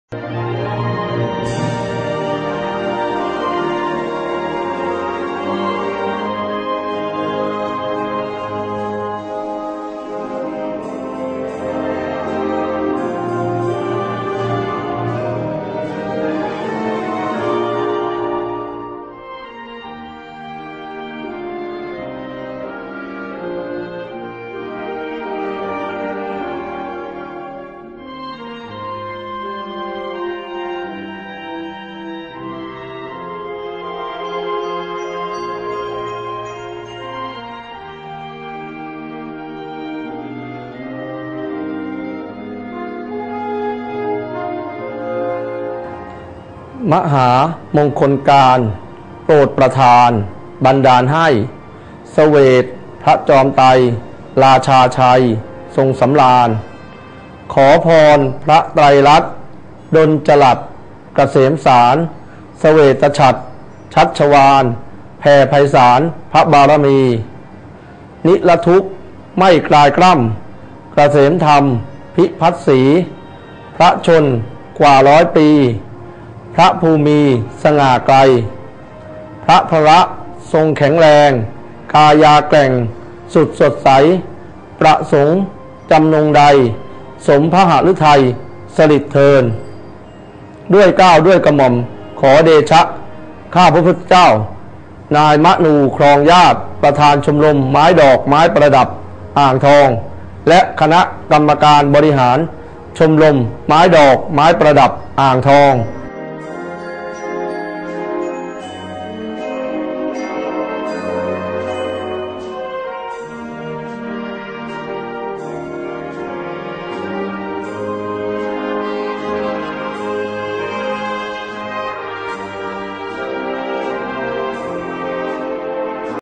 5-12-54_41_ชมรมไม้ดอกไม้ประดับอ่างทอง
5 ธันวาคม 2554 : ทีฆายุกา โหตุ มหาราชา
ด้วยเกล้าด้วยกระหม่อมขอเดชะ ข้าพระพุทธเจ้า
มะนู ครองญาติ ประธานชมรมไม้ดอกไม้ประดับอ่างทอง